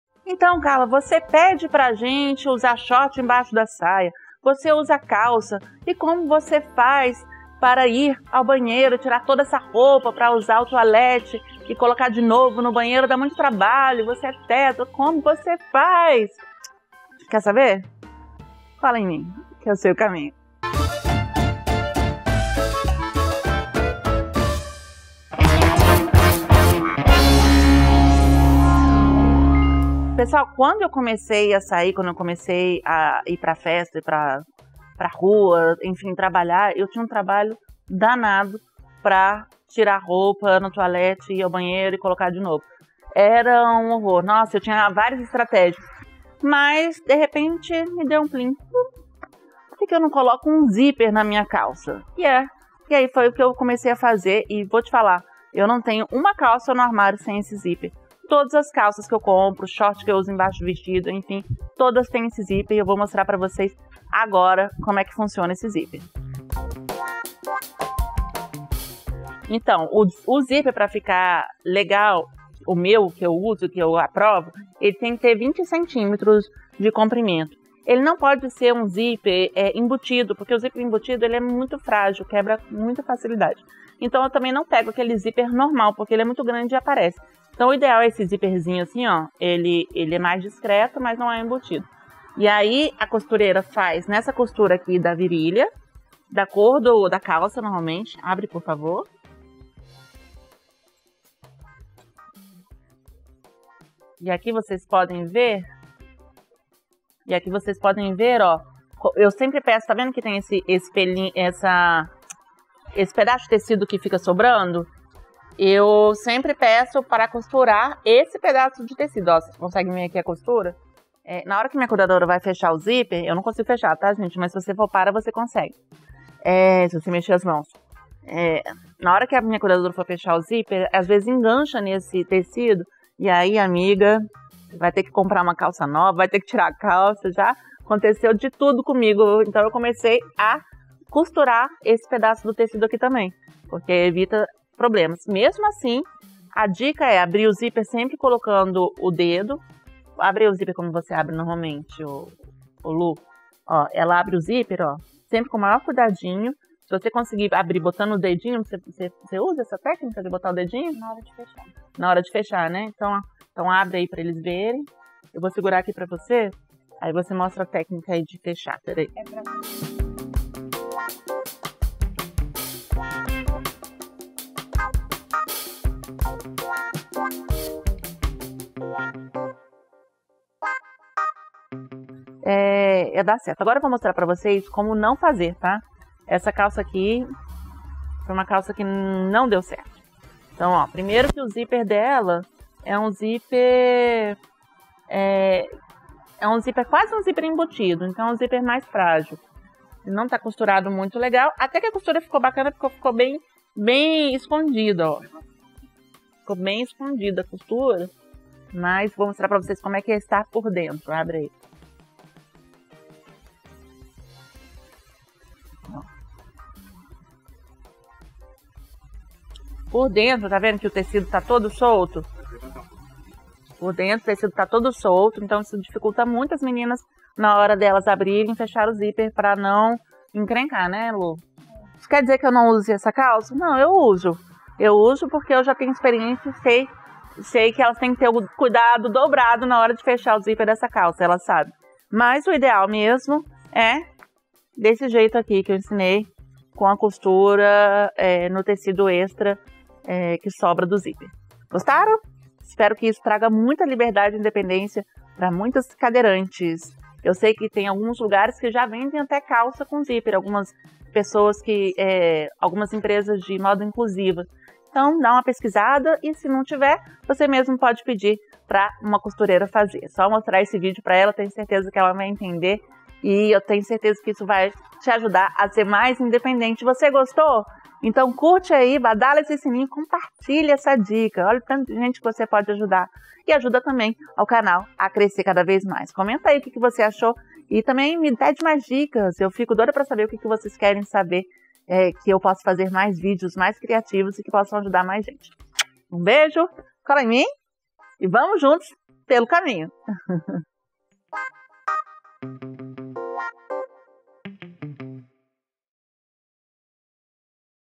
Adaptação para calça??? Funciona?
Pessoal hoje tenho uma dica muito bacana! Pra você que é cadeirante e quer saber como ir algo banheiro de uma forma mais prática se liga nessa calça adaptada que você também pode fazer por aí!